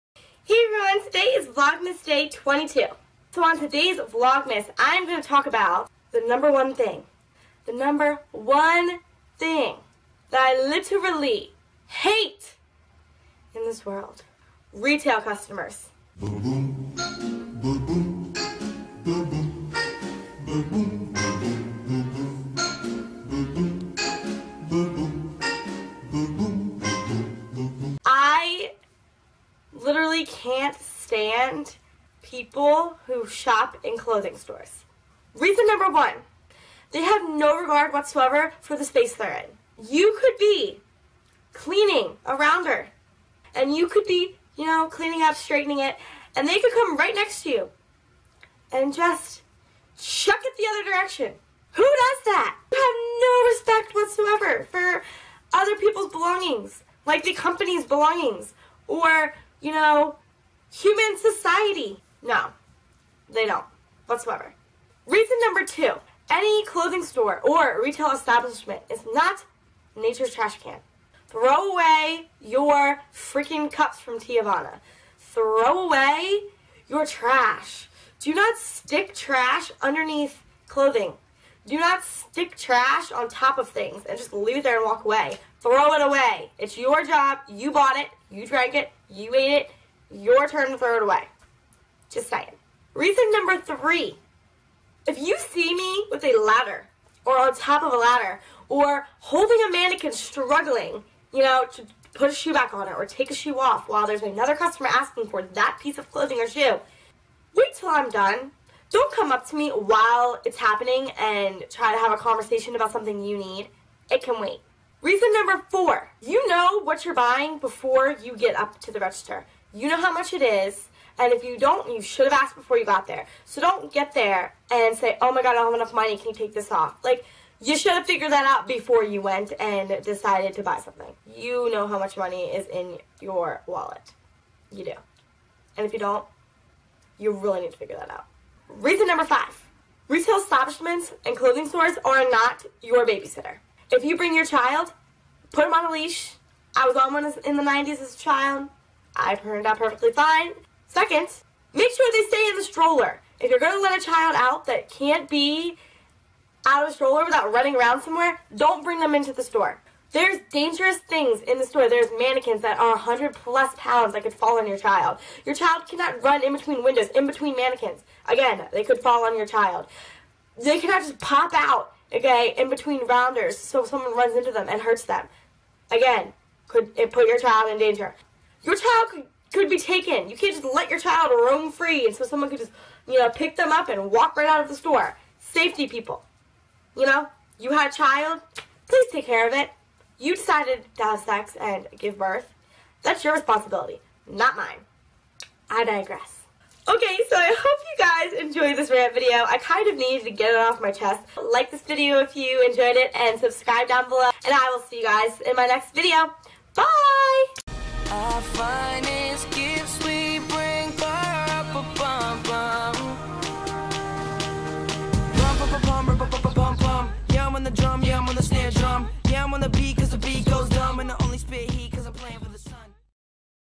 ❄5 Reasons I Can't Stand Retail Customers│Vlogmas Day 22! ❄
Hey beauties!!! Today is Vlogmas Day 22! I am so excited to finally participate in this! Today I have a rant video haha! I explain my annoying thoughts about retail customers! Tis the season! ;) XO

Stalk me!

XO,
Jenny